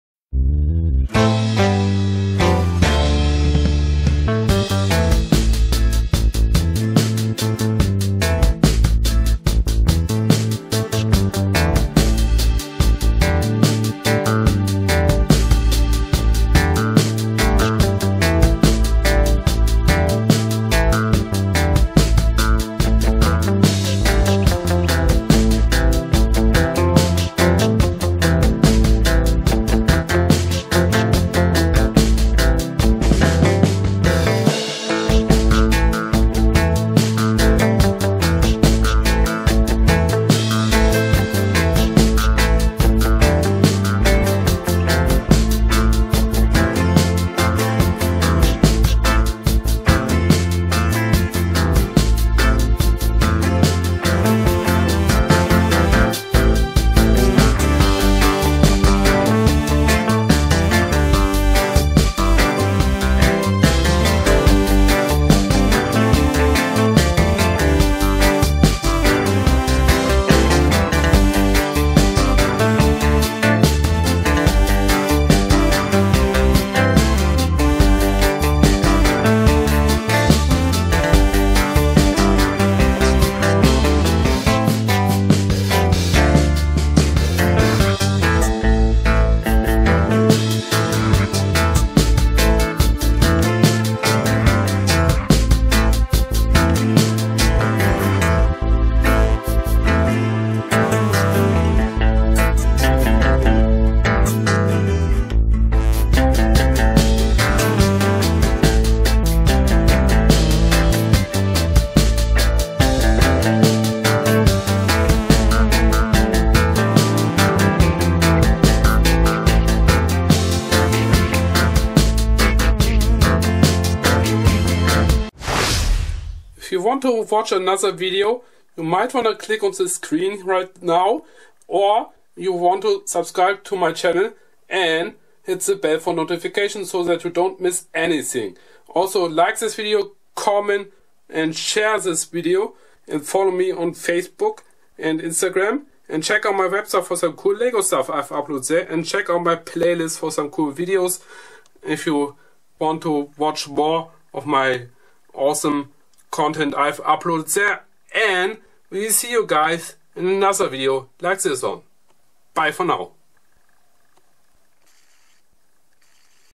LEGO Town Street Sweeper Set 6649 Stopmotion
This is my stopmotion of LEGO® set 6649 Street Sweeper from LEGO® Town...

Informations:

Name: Street Sweeper
Number: 6649
Theme: Town
Minifigures: 1
Pieces: 63
Released: 1995